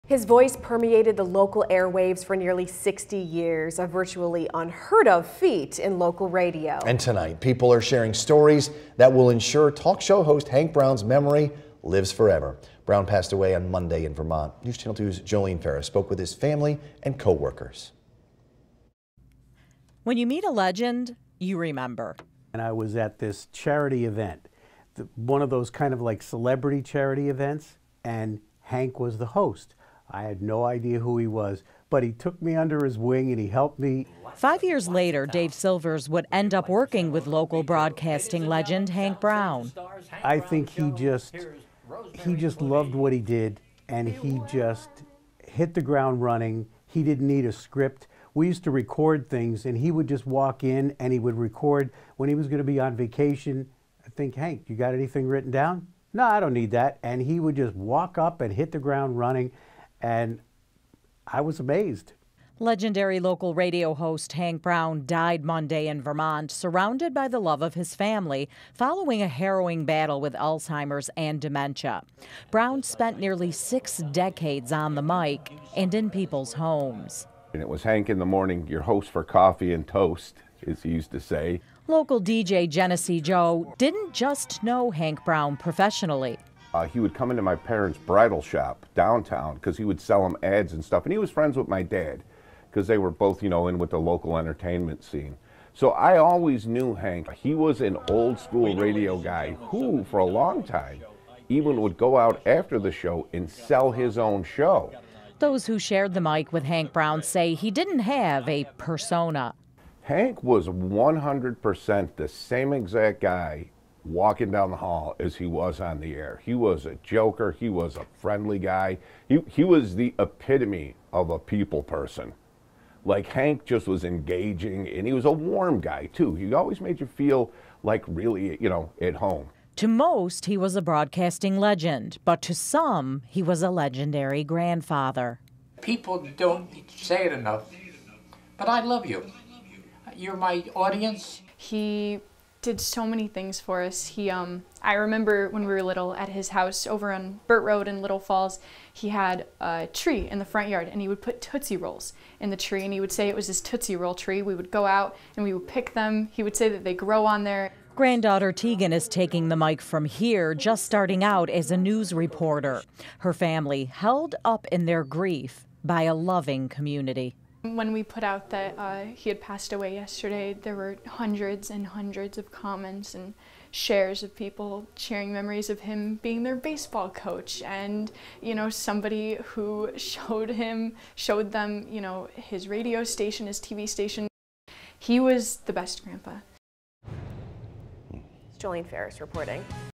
Local broadcaster Hank Brown remembered
The community remembered Hank Brown, a local broadcaster for nearly 60 years, after he passed away on Monday, May 9, 2022.